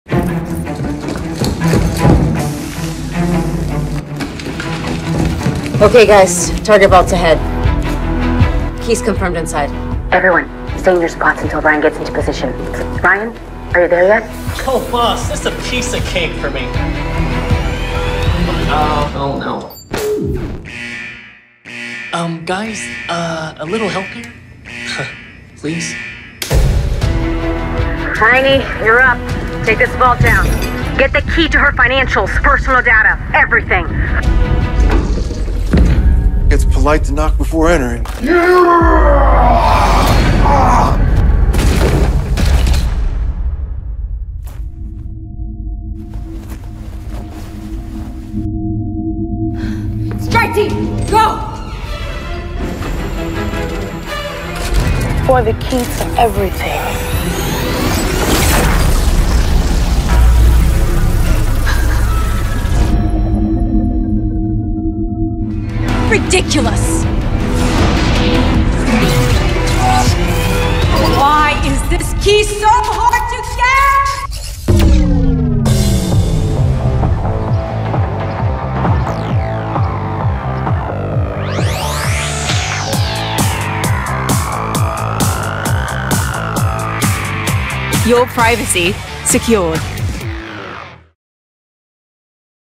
الخصوصية مع سامسونج: جوال Galaxy S23، THE VAULT
نظام Knox Vault
يضمن لك الحماية على مفتاح الأمان الخاص فيك، ويحافظ على حماية بياناتك الشخصية
اعرف المزيد: